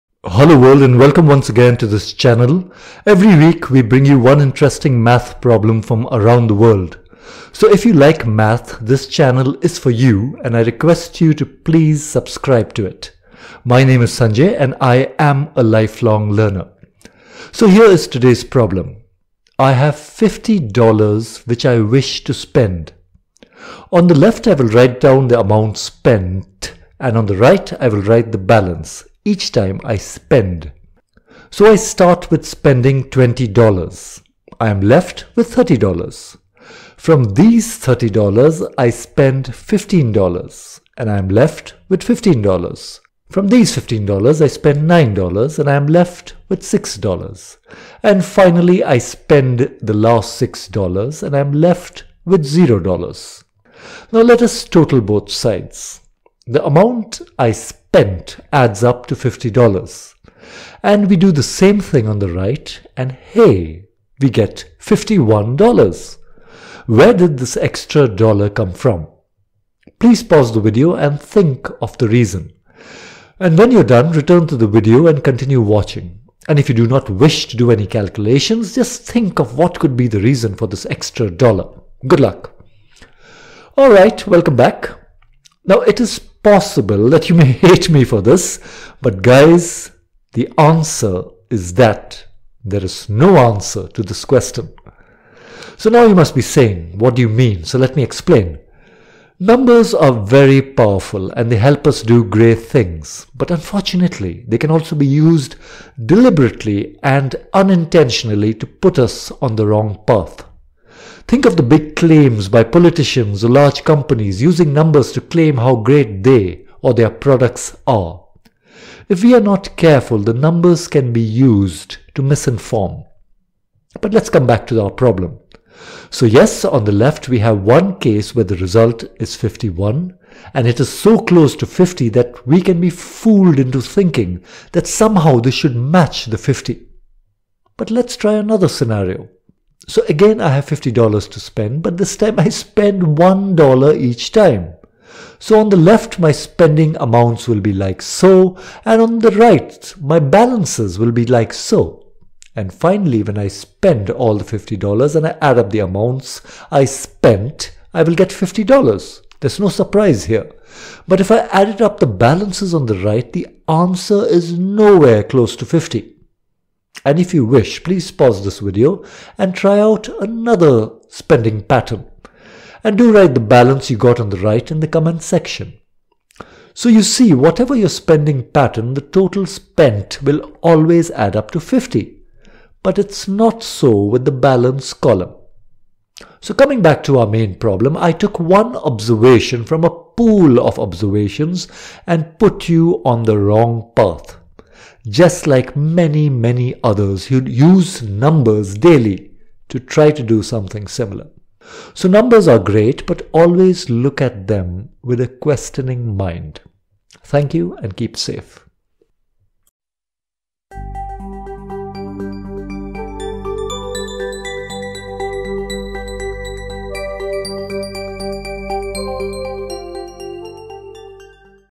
This is a practical Math problem. Can you solve it?
Numbers are very powerful and they help us do great things.
But unfortunately, they can also be used, deliberately and un-intentionally do put us on the wrong path .
Hello world and welcome my channel.
Every week we bring you one interesting Math problem from around the world.
So can you find where did the extra dollar come from?